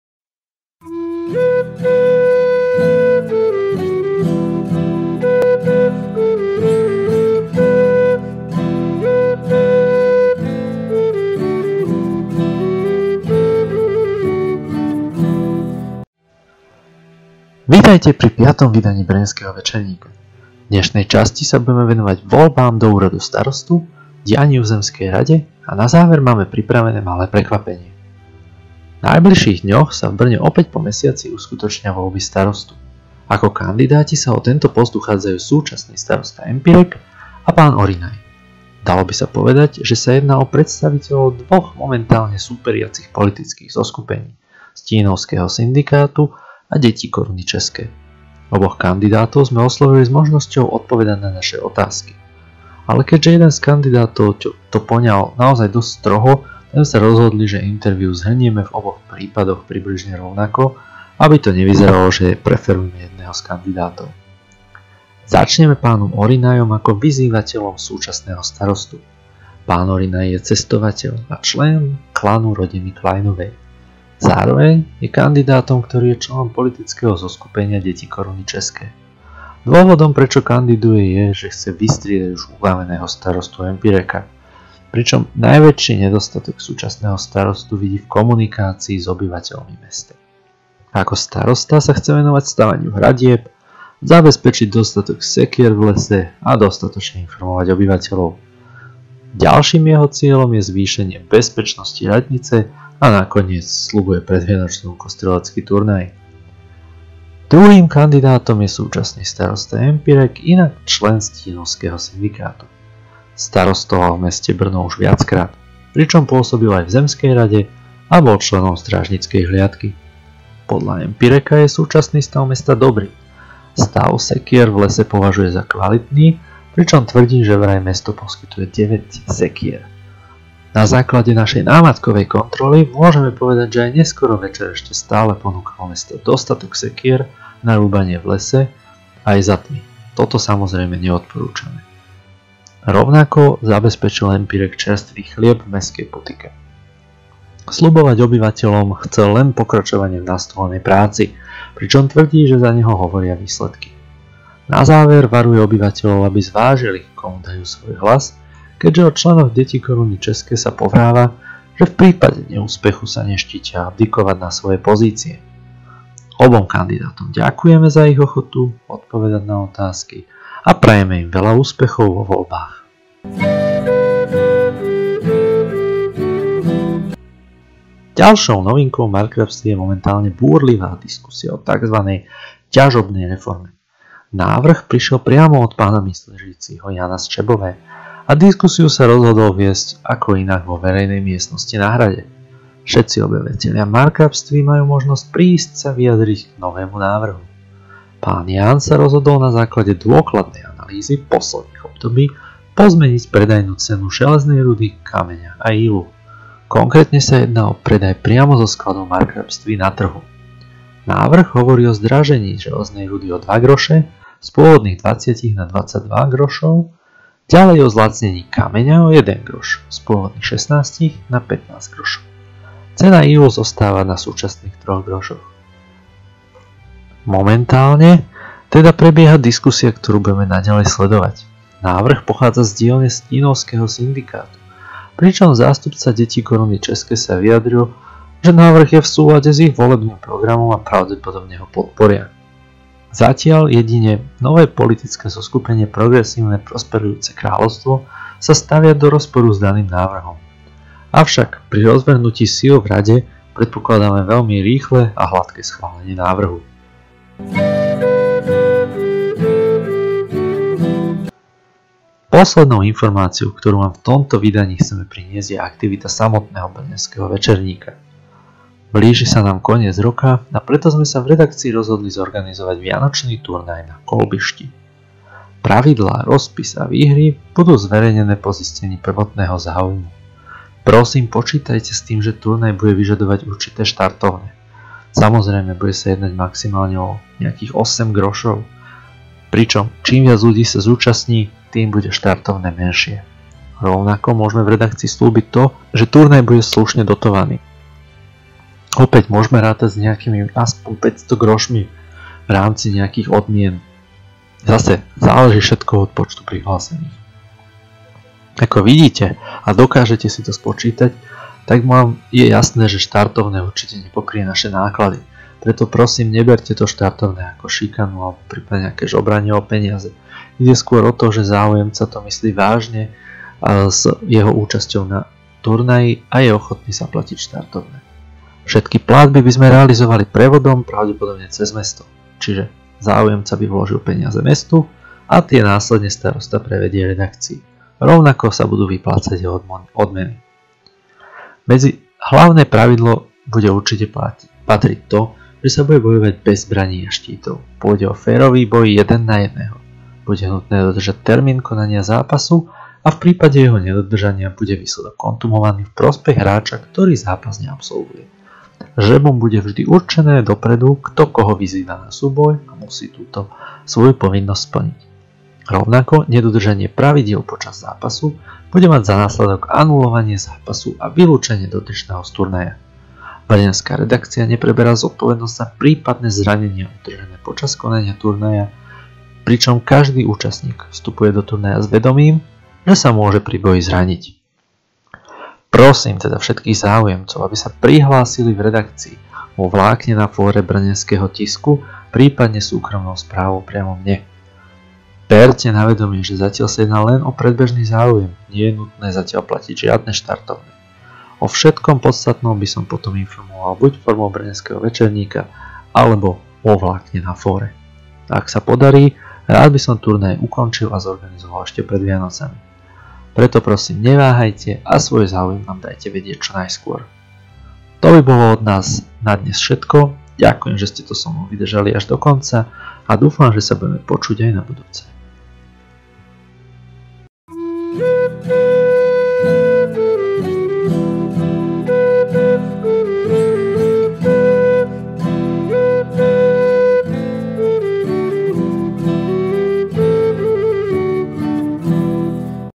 Brnenský večerník V - RK [podcast]
Vitajte pri piatom vydaní Brnenského Večerníka. V dnešnej časti sa budeme venovať voľbám do úradu starostu, dianiu v zemskej rade a na záver máme pripravené malé prekvapenie.
Luboš